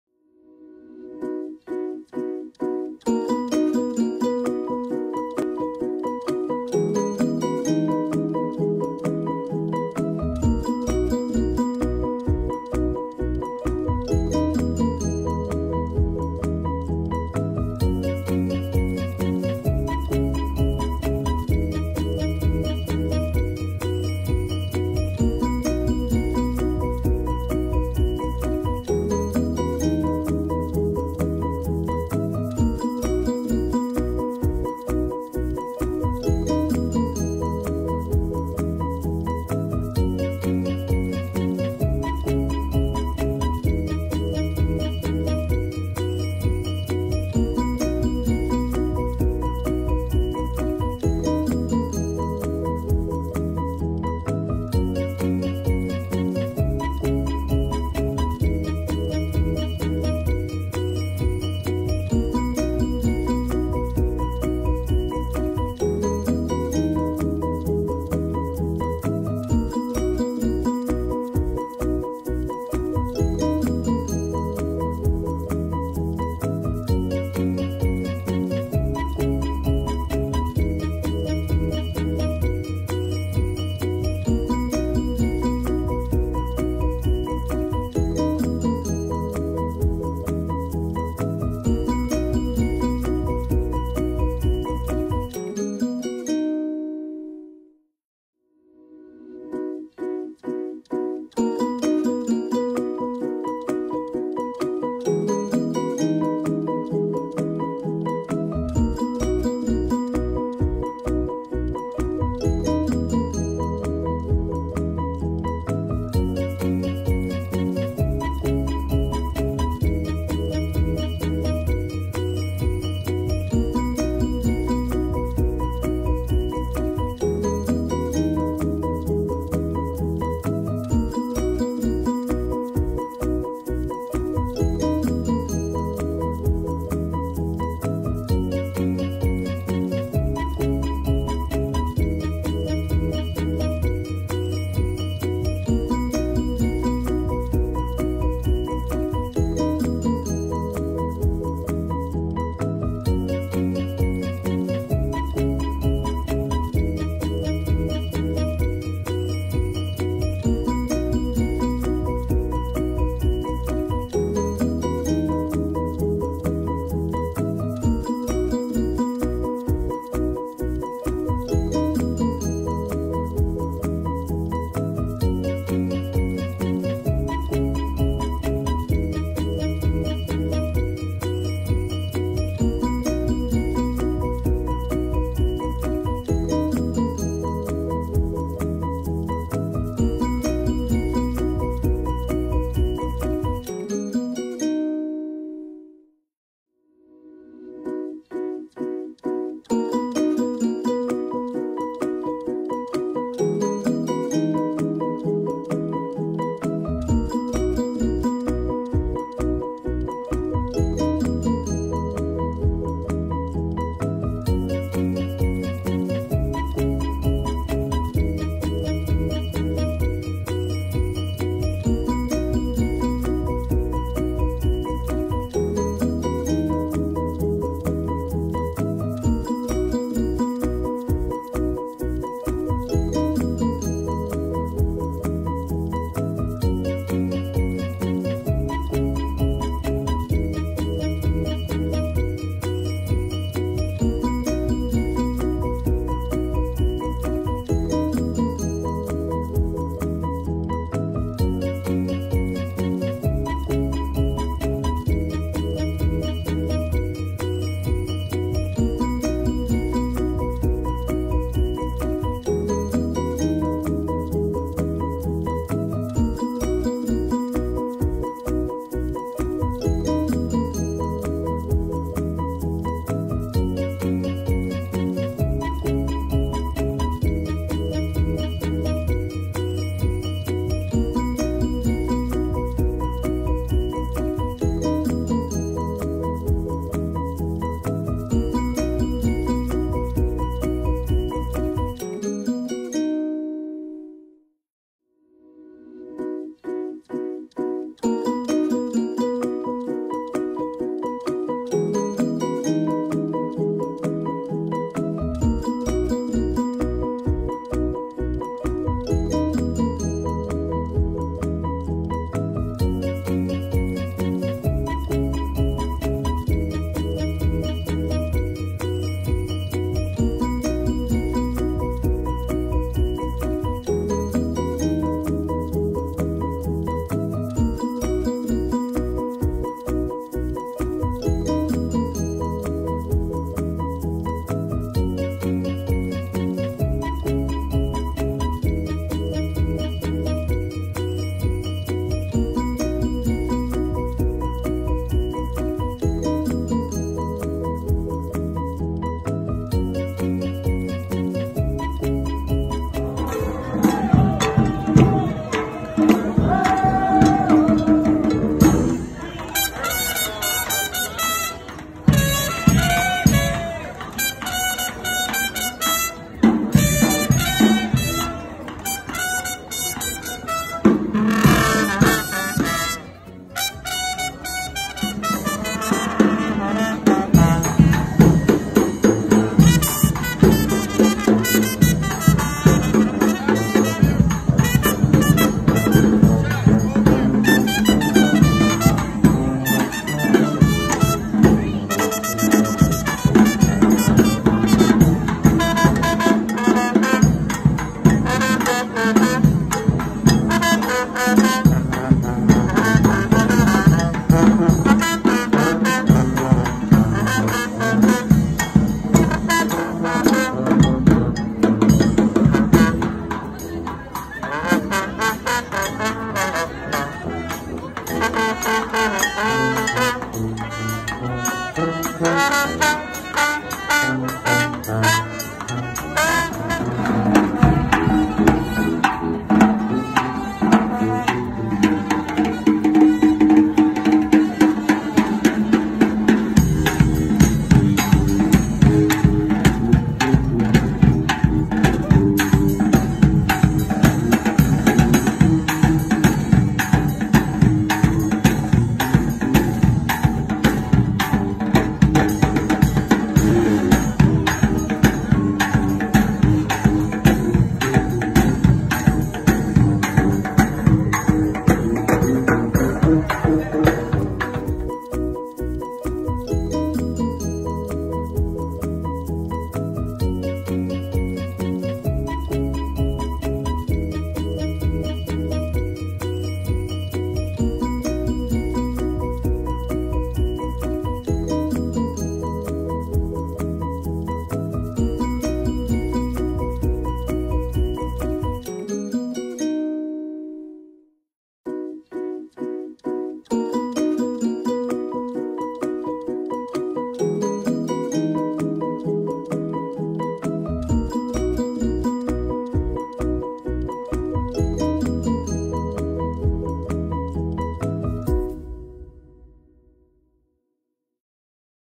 New Orleans (NOLA) 2021
I went on a short trip to New Orleans for Spring break this year. There’s so much to see and do. Here’s a few things I did while visiting New Orleans.